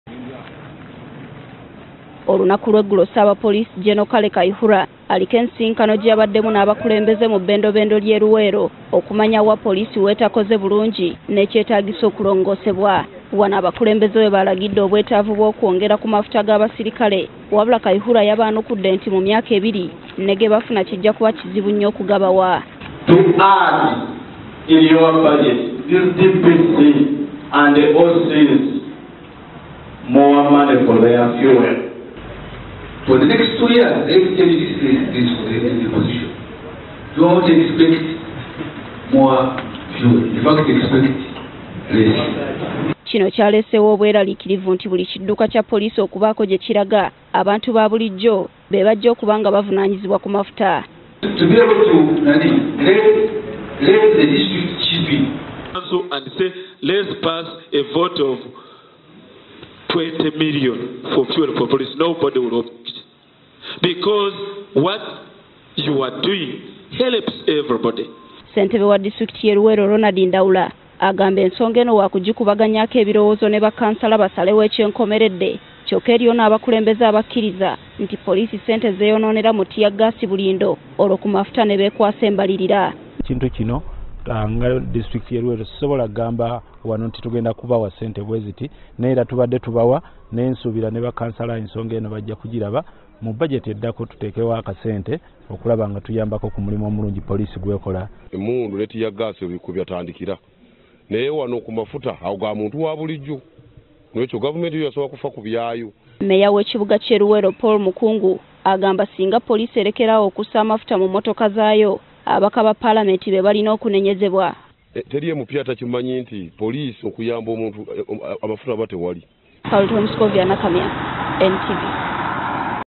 Okuwa Poliisi amafuta: Abakulembeze baanukudde Gen. Kayihura
Olunaku lweggulo ssabapoliisi Gen Kale Edward Kayihura yasinziddemu nsisinkano n’abakulembeze mu bendobendo lye Luweero nabasaba mumbalirira zabwe okussaamu ssente z’amafuta aganaakola emirimu gyapoliisi kubanga bbo tebakyalina nsimbi.Abakulembeze be Luweero NTV beyogeddeko nabo abamu bakkiriza ntikyetaagisa wabula abamu bamusabye akendeeze ku ssente zassa mu motokaza tear gas ziteekebwe ku mafuta.